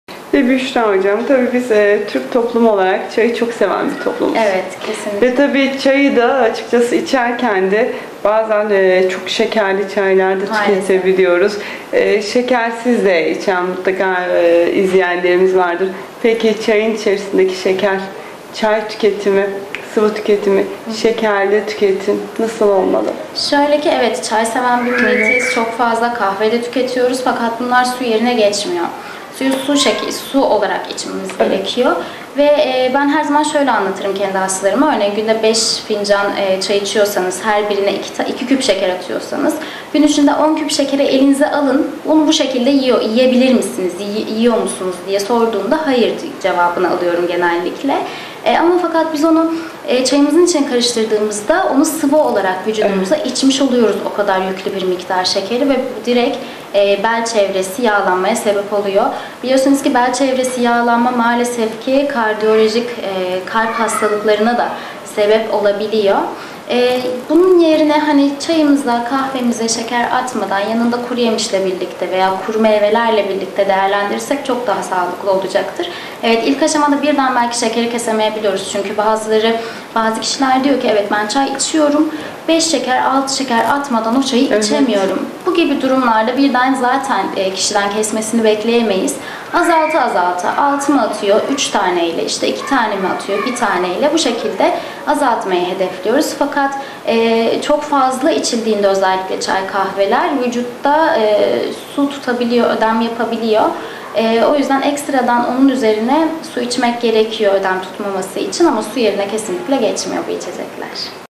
Şeker Tüketimi Nasıl Olmalı ?
Avrasya Hastanesi Uzman Diyetisyen Büşra Çengel '' Şeker Tüketimi Nasıl Olmalı ? '' hakkında detayı bilgiler veriyor.